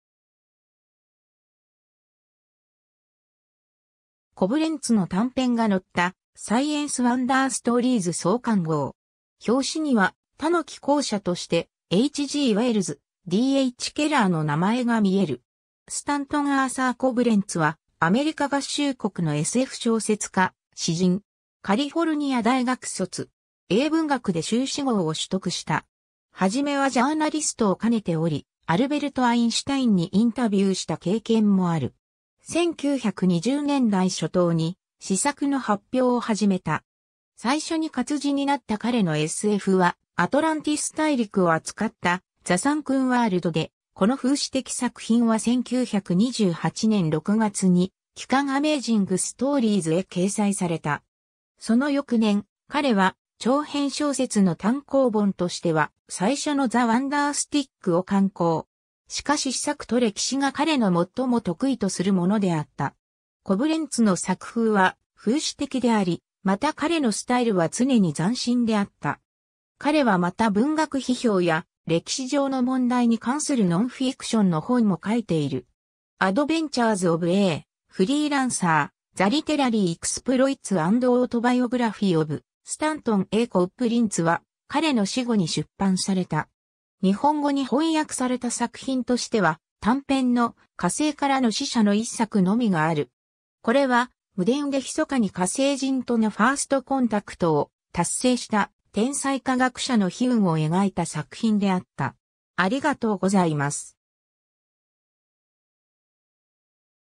スタントン・A・コブレンツ
コブレンツの短編が載った『サイエンス・ワンダー・ストーリーズ』創刊号（1929年6月号）。
表紙には、他の寄稿者としてH・G・ウェルズ、D・H・ケラーの名前が見える。
 スタントン・アーサー・コブレンツ（Stanton Arthur Coblentz、1896年8月24日 - 1982年9月6日）はアメリカ合衆国のSF小説家、詩人。
カリフォルニア大学卒。
英文学で修士号を取得した。
 はじめはジャーナリストを兼ねており、アルベルト・アインシュタインにインタビューした経験もある。
1920年代初頭に詩作の発表を始めた。
最初に活字になった彼のSFは、アトランティス大陸を扱った"The Sunken World"（沈んだ世界）で、この風刺的作品は1928年6月に『季刊アメージング・ストーリーズ』（Amazing Stories Quarterly）へ掲載された。
その翌年、彼は長編小説の単行本としては最初の"The Wonder Stick"（びっくり棒）を刊行。
しかし詩作と歴史が彼の最も得意とするものであった。
コブレンツの作風は風刺的であり、また彼のスタイルは常に斬新であった。
彼はまた文学批評や歴史上の問題に関するノンフィクションの本も書いている。
"Adventures of a Freelancer: The Literary Exploits and Autobiography of Stanton A. Coblentz"（あるフリーランサーの冒険 - スタントン・A・コブレンツの文学的業績および自叙伝）は彼の死後に出版された。
 日本語に翻訳された作品としては、短編の「火星からの使者」（Missionaries from the Sky）の一作のみがある（「SFマガジン」1978年増刊号：178号）。
これは、無電で密かに火星人とのファースト・コンタクトを達成した天才科学者の悲運を描いた作品であった。